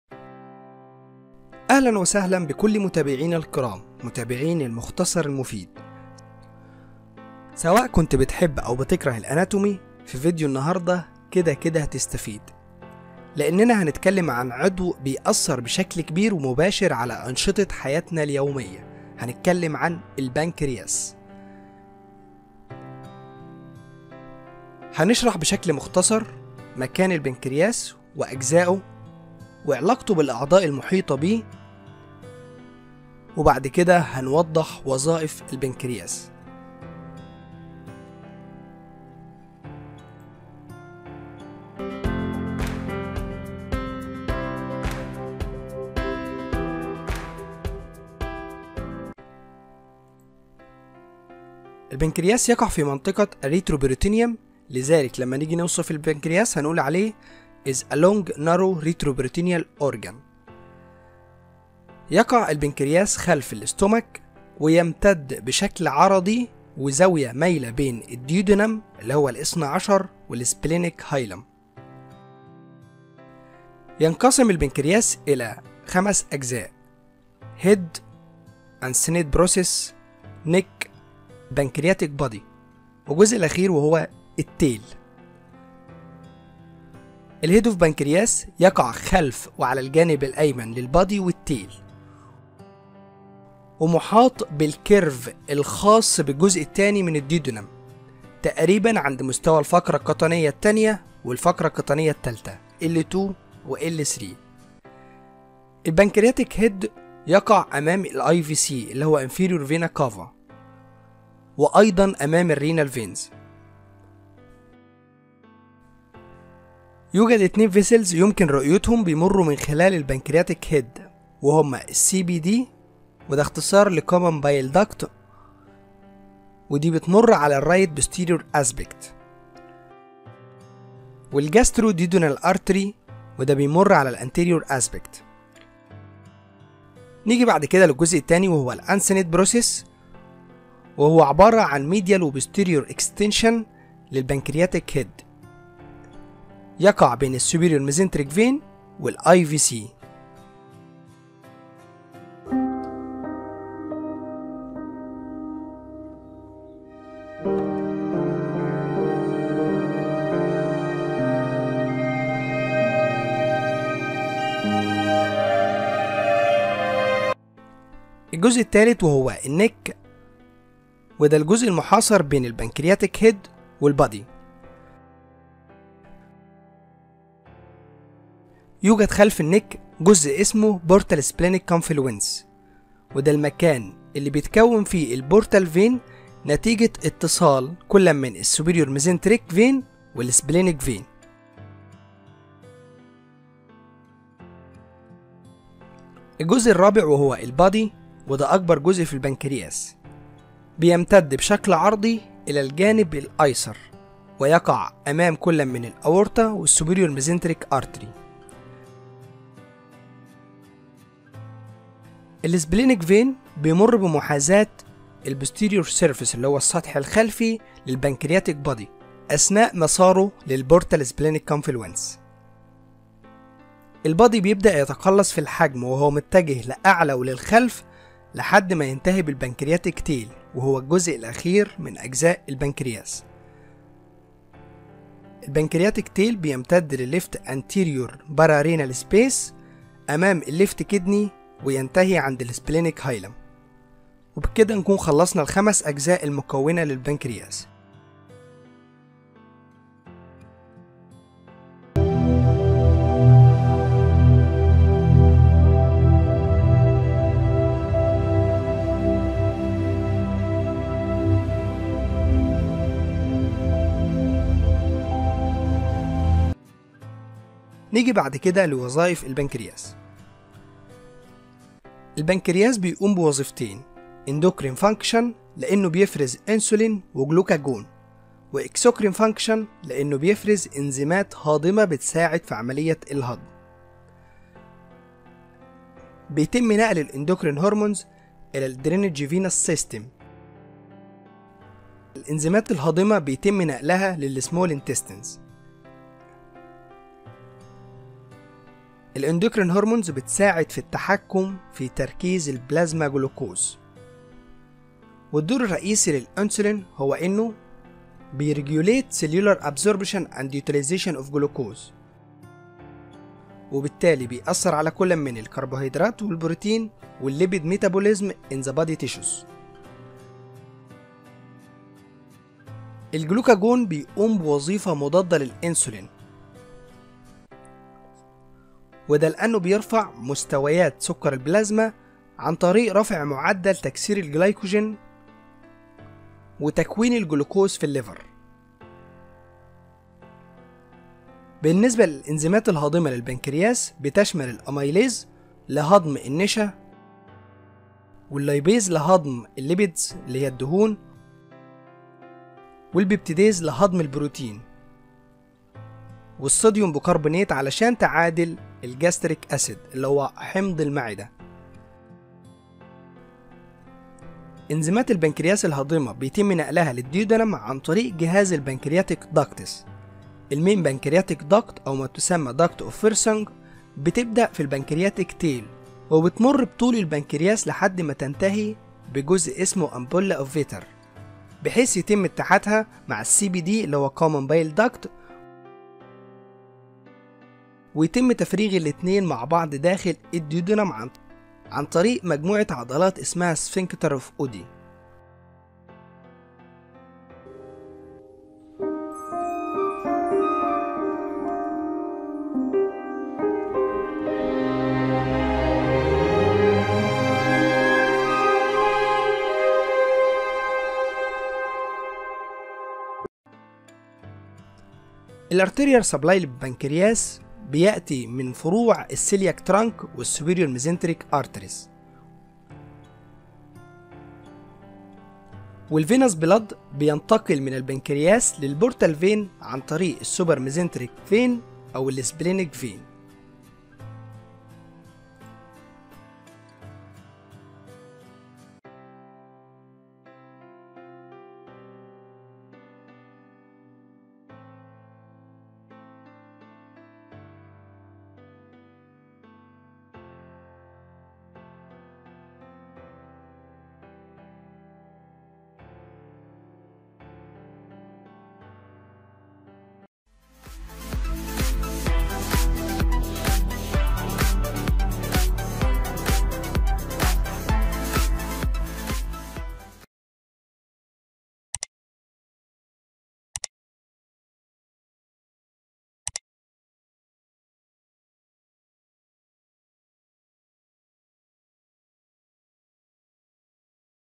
البنكرياس pancreas
Anatomy and physiology of the pancreas
تشريح البنكرياس
شرح وظائف البنكرياس
شرح وظيفه الأنسولين والجلوكاجون